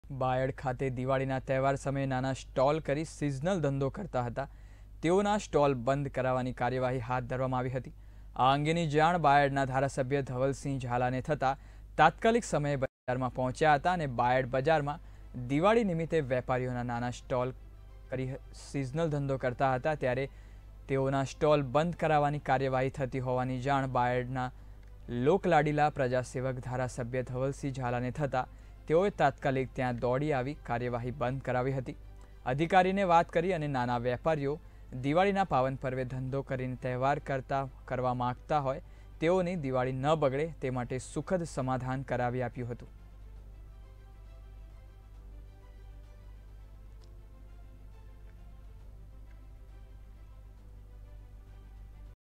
ARVLLI NEWS: બાયડમાં દિવાળી સમયે સ્ટોલ બંધ કરાવાની કાર્યવાહી હાથ ધરી
બાયડમાં દિવાળી સમયે સ્ટોલ બંધ કરાવાની કાર્યવાહી હાથ ધરી
ધારાસભ્ય ધવલસિંહ ઝાલા બજારની મુલાકાતે પહોંચ્યા
દિવાળી ન બગડે તે માટે સુખદ સમાધાન કરી આપ્યું